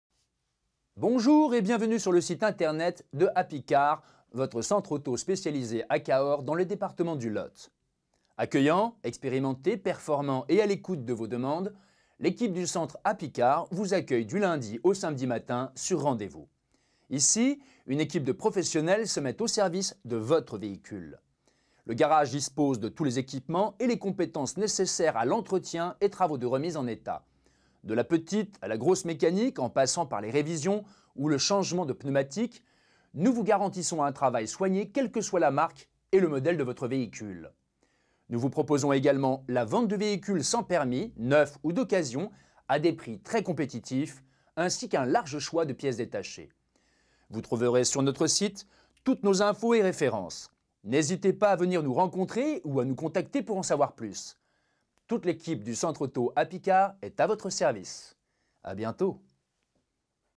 Entretien et réparation automobiles à Cahors (46) : HAPPY CAR
Happy Car, votre centre auto spécialisé à Cahors dans le département du Lot.
Accueillants, expérimentés, performants et à l'écoute de vos demandes, l'équipe du centre Happy Car vous accueille du lundi au samedi matin, sur rendez-vous. Ici, une équipe de professionnels se met au service de votre véhicule.

Le garage dispose de tous les équipements et les compétences nécessaires à l'entretien et travaux de remise en état. De la petite à la grosse mécanique en passant par les révisions, ou le changements de pneumatiques, nous vous garantissons un travail soigné quel que soit la marque et le modèle de votre véhicule.

Nous vous proposons également la vente de véhicules sans permis, neufs ou d'occasion à des prix très compétitifs ainsi qu'un large choix de pièces détachées.

Vous trouverez sur notre site toutes nos infos et références. N'hésitez pas à venir nous rencontrer ou à nous contacter pour en savoir plus. Toute l'équipe du centre auto Happy Car est à votre service.